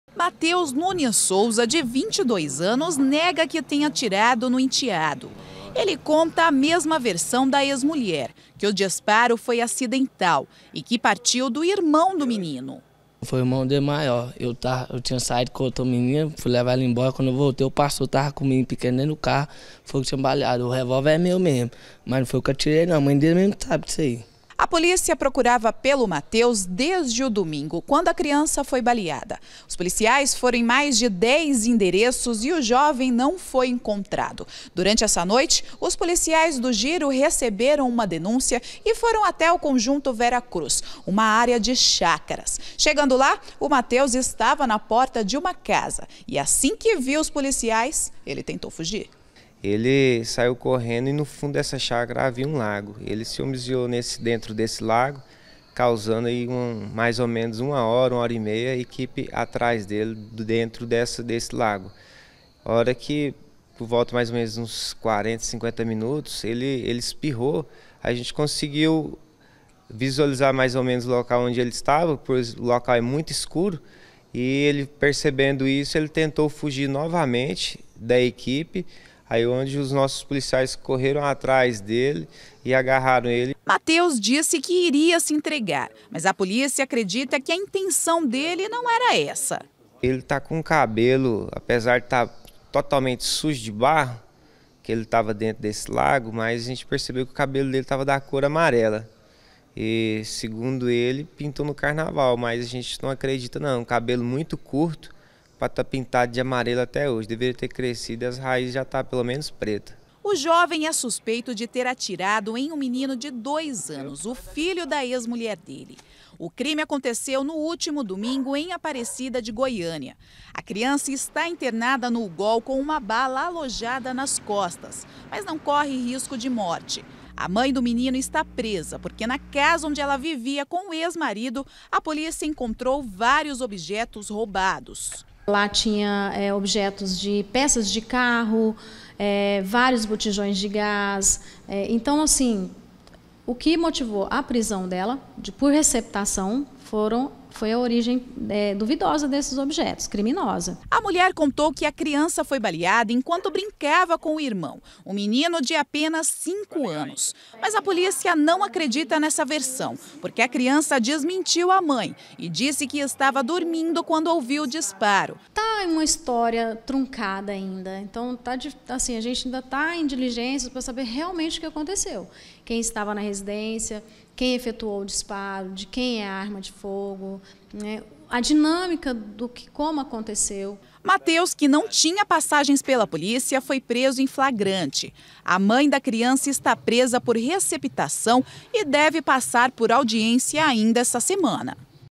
PRESO O SUSPEITO DE ATIRAR EM CRIANÇA: EX-PADRASTO NEGA O CRIME
Foi preso o homem suspeito de ter atirado em uma criança de dois anos, em Aparecida de Goiânia. Ele que é ex-padrasto da do menino nega o crime.